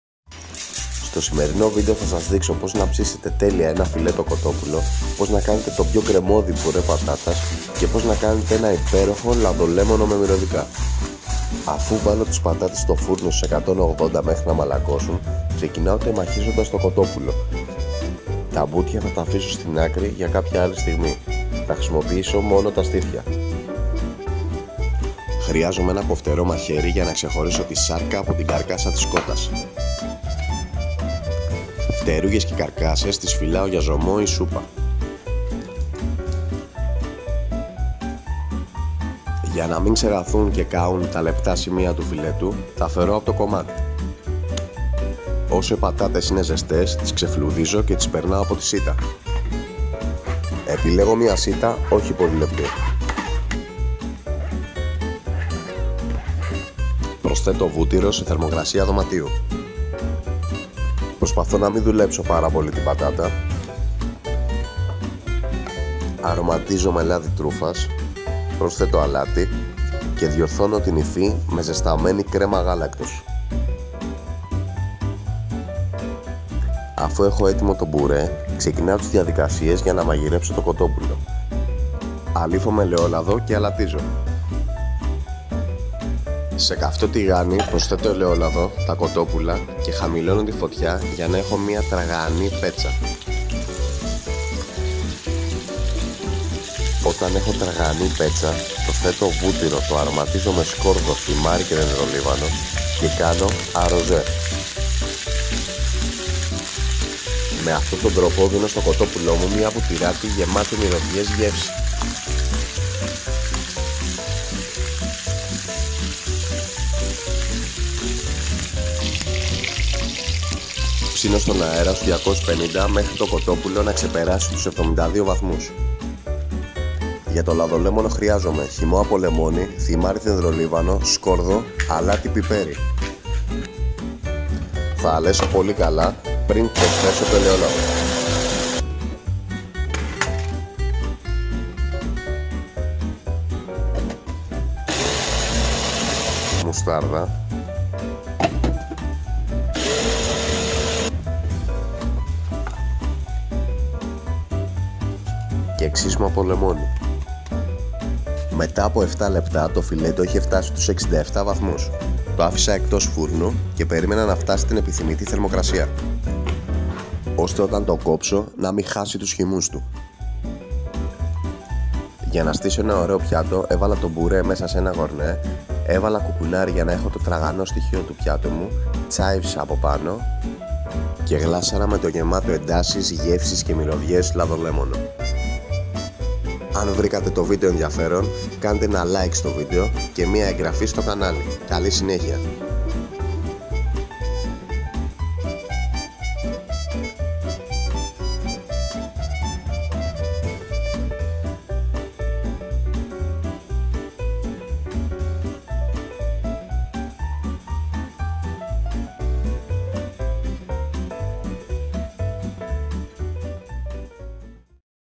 Φιλέτο κοτόπουλο, κρεμώδης πουρές πατάτας, λαδολέμονο με μυρωδικά | Μίντζηρας Θεμιστοκλής
Απλότητα...
Φιλέτο κοτόπουλο μαγειρεμένο πρώτα στο τηγάνι και στη συνέχεια στο φούρνο, κρεμώδης πουρές πατάτας αρρωματισμένος με λάδι τρούφας και ένα γεμάτο εντάσεις και μυρωδιές λαδολέμονο!

Πουρές πατάτας:
πατάτες 1 κιλό
βούτυρο 180 γρ
κρέμα γάλακτος 180γρ περίπου (ανάλογα και με την υγρασία της πατάτας, μέχρι να έχει καλή υφή)
αλάτι
λάδι τρούφας

Λαδολέμονο:
χυμό λεμόνι 70 γρ
θυμάρι 1 πρέζα
δενδρολίβανο 1 πρέζα
μουστάρδα dijon 1 κτσ
σκόρδο 1 σκελίδα
αλάτι
μαύρο πιπέρι
ελαιόλαδο 180 γρ

*Βάζω το θερμόμετρο στο πιο χοντρό σημείο του φιλέτου, το κοτόπουλο είναι έτοιμο όταν η θερμοκρασία ξεπεράσει τους 72 με 75 βαθμούς κελισίου